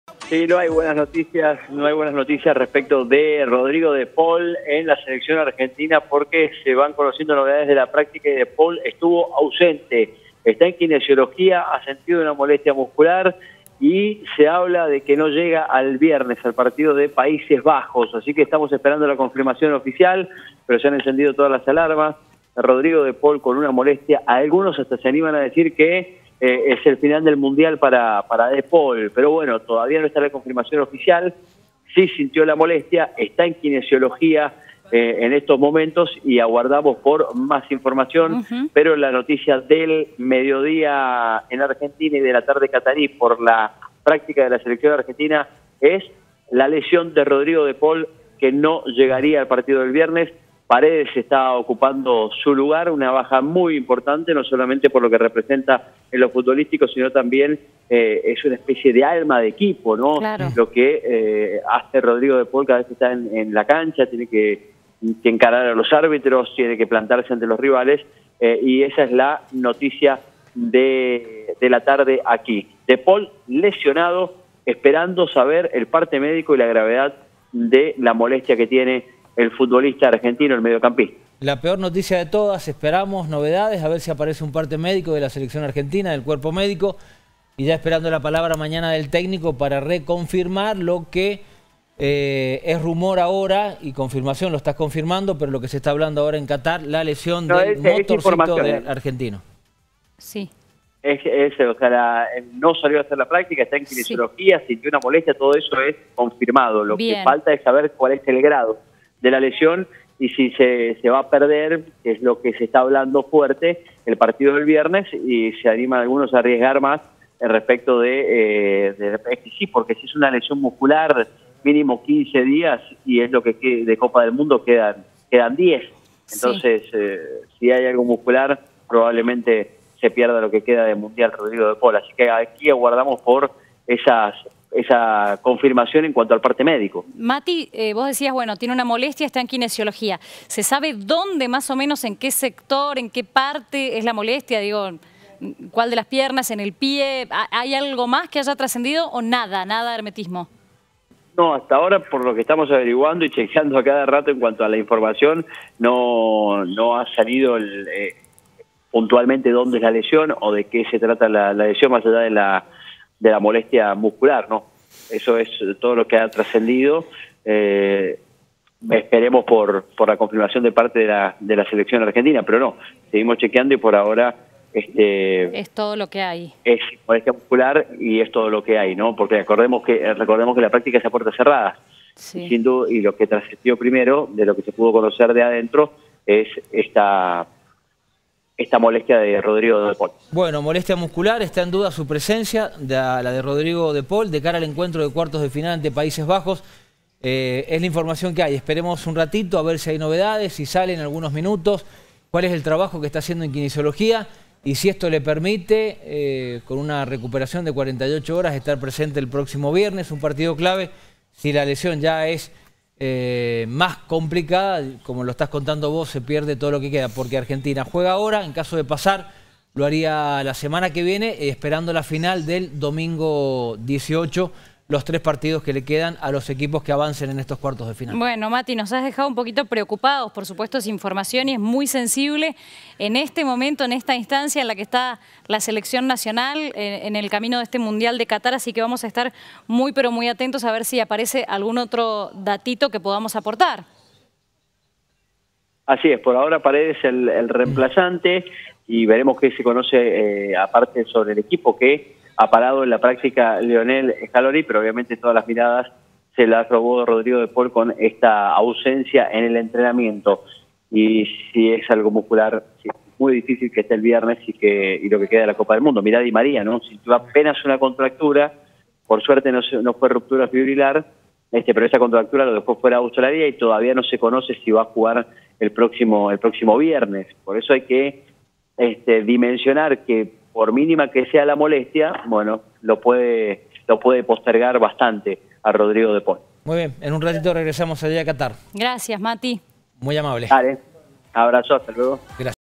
ALARMA CON DE PAUL I No jugaría el viernes por una molestia muscular
Titular en los cuatro partidos de la Selección argentina en el Mundial Qatar 2022, De Paul padece una molestia muscular en el isquiotibial por la que no se entrenó, estuvo trabajando en kinesiología junto al Papu Gómez (tiene un esguince de tobillo), fue sometido a estudios y desde el cuerpo médico creen que es difícil que pueda jugar este viernes.